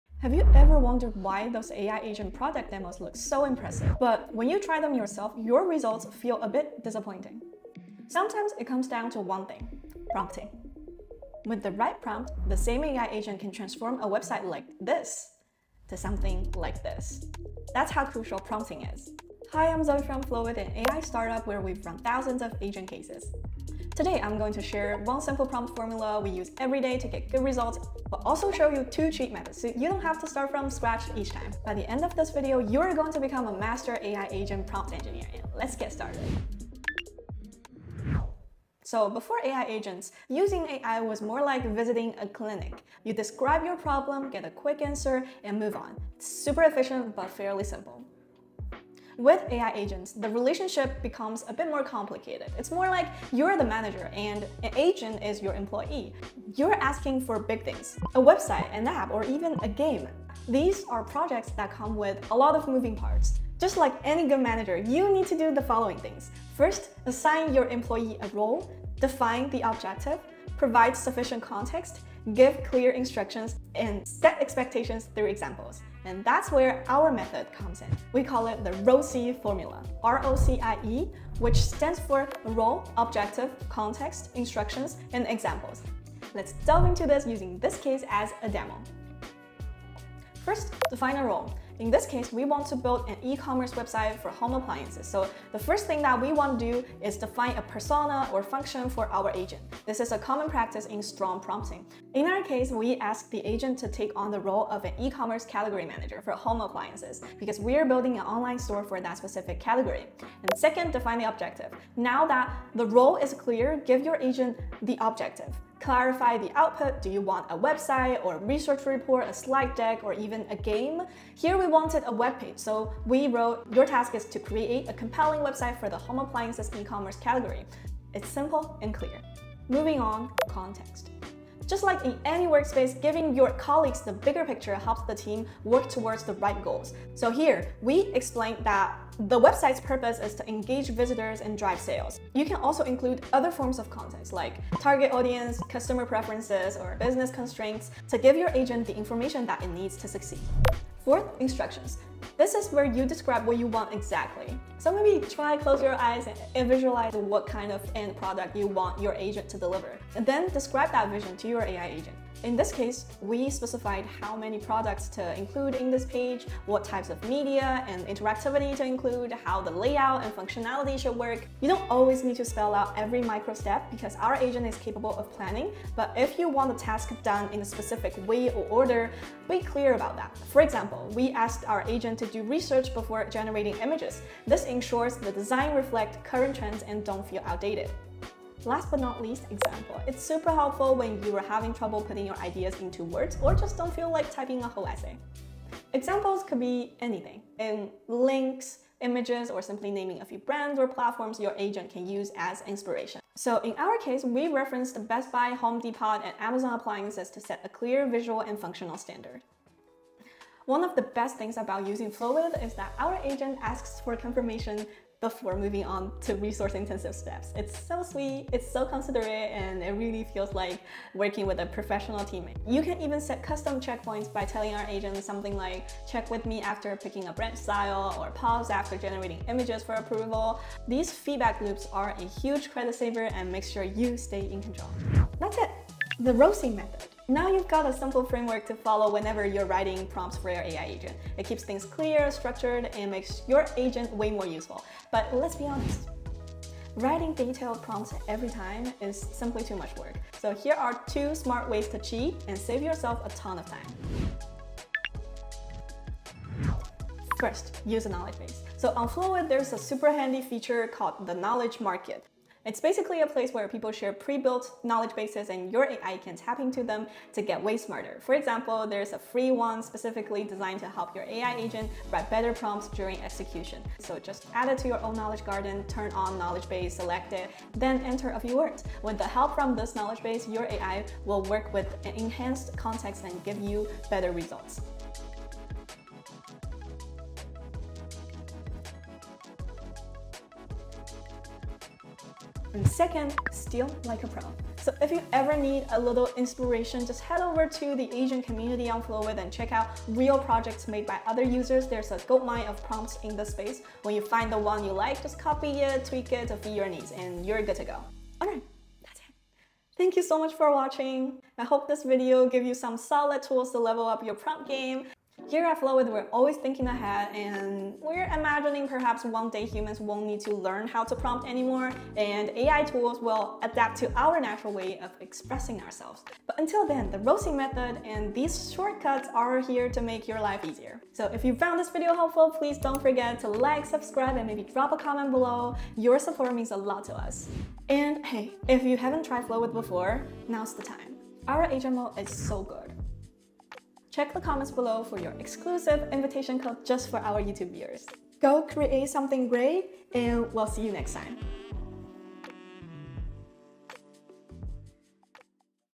The secret formula for prompting AI Agents (from a real AI start-up)
In this video, we share R-O-C-I-E, the 5-step framework our start-up uses every day to make our AI agent work smarter and more effectively.